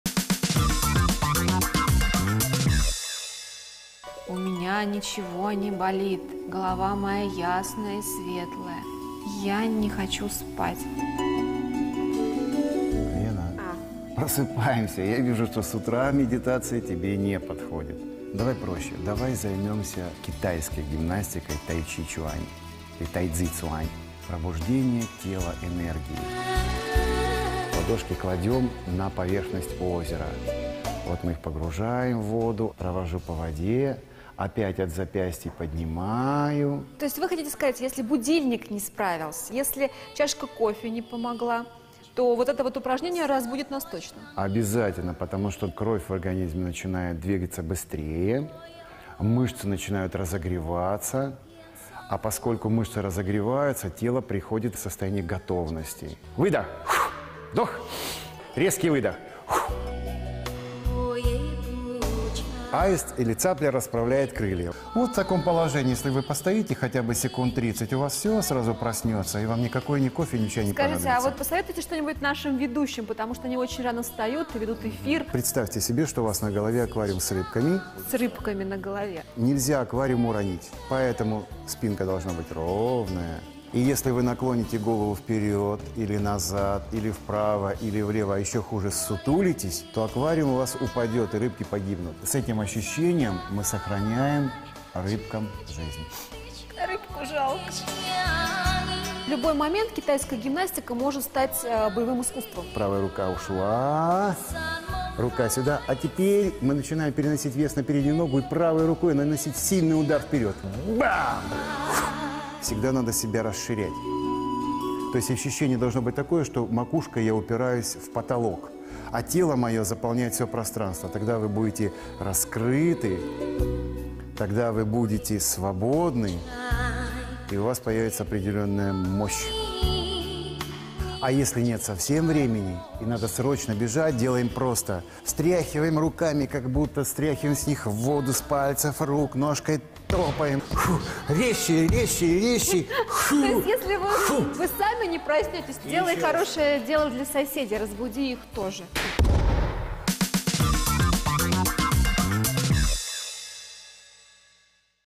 Китайская гимнастика тайцзицюань – пробуждение тела энергией. "Тело в дело!"
Если с утра от медитации ещё больше тянет в сон, то остаётся один выход – гимнастика тайцзицюань. Что хорошо для китайца, то и русскому не во вред.